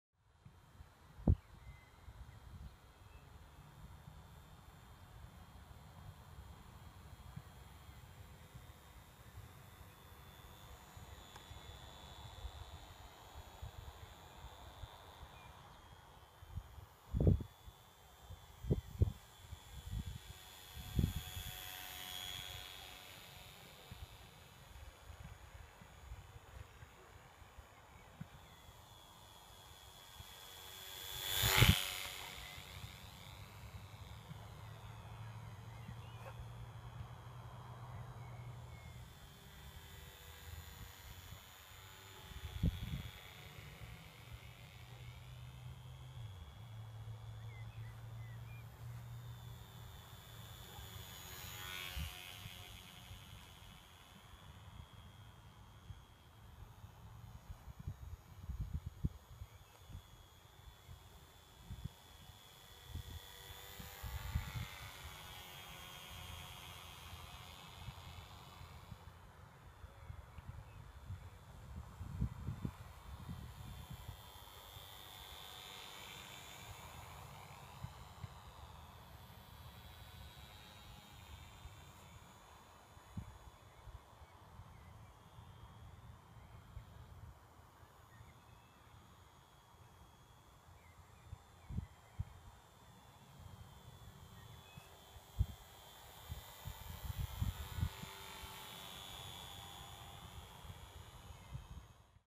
Evening Patrol with the Microaces Bristol F.2b
A light wind evening great for flying sedate and slow flying micros!